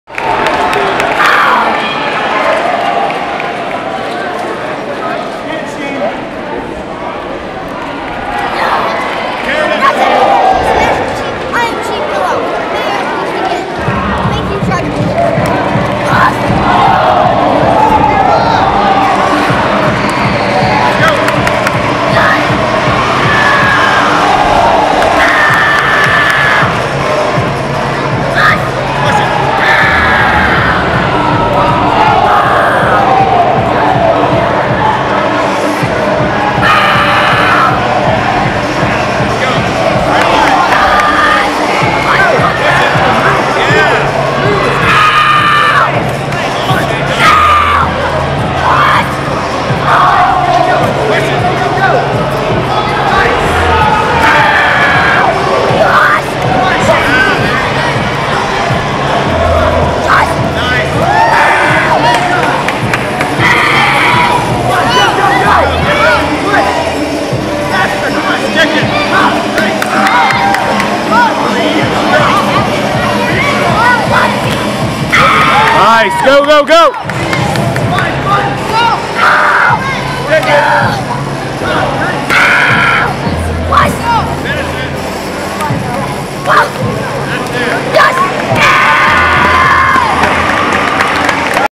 Shane Billow - creative forms
Shane Billow creative forms Amerikick Internationals 2016; karate; NASKA world top 4 creative forms, creative weapons and extreme weapons; amerikick national team member